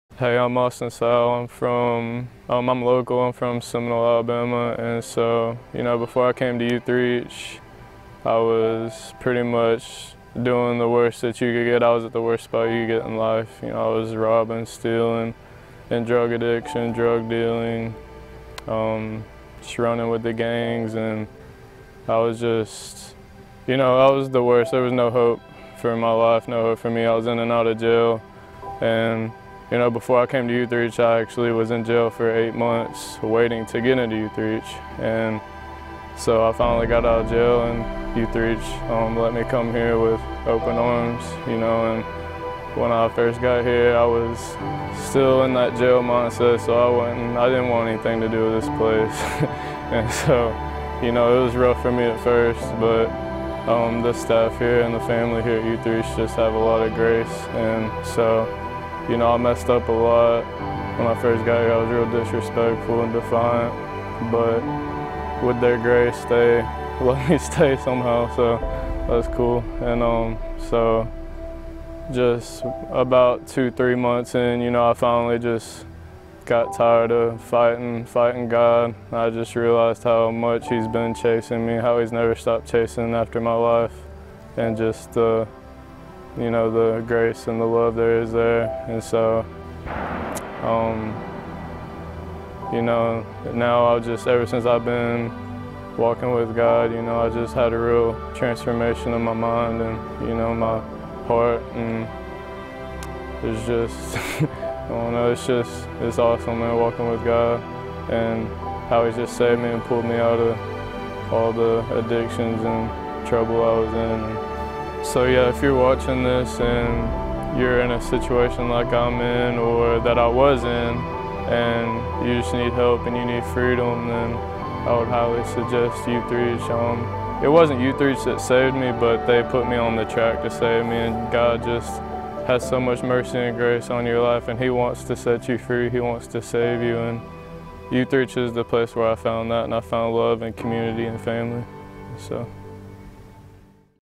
Austin's Testimony
Although he didn't want anything to do with Youth-Reach at first, Austin finally got tired of fighting God after his first couple of months and surrendered to the grace and love available through a relationship with Christ.